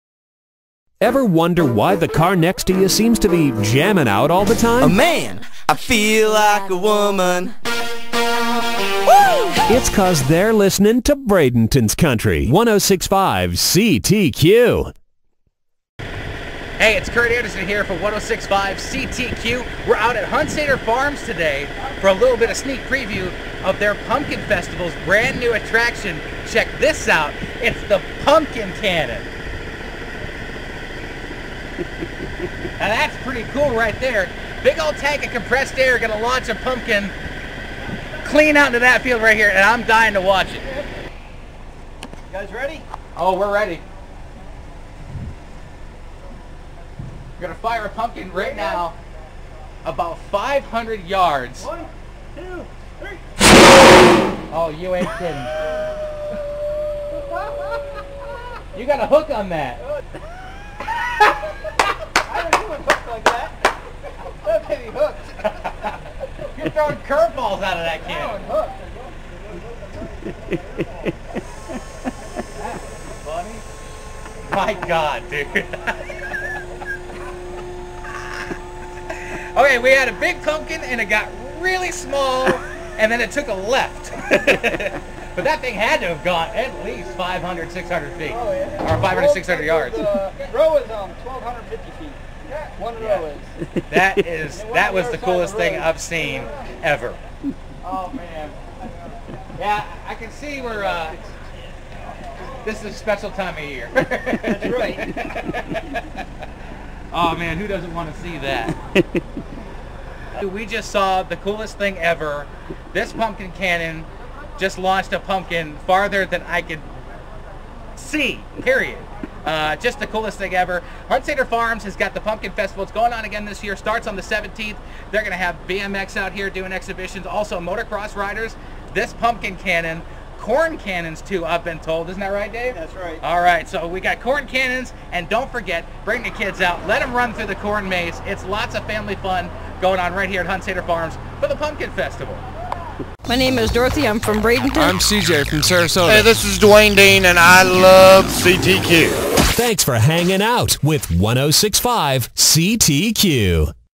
Pumpkin Cannon!!!
A huge compressed air cannon that launches pumpkins over 1300 feet.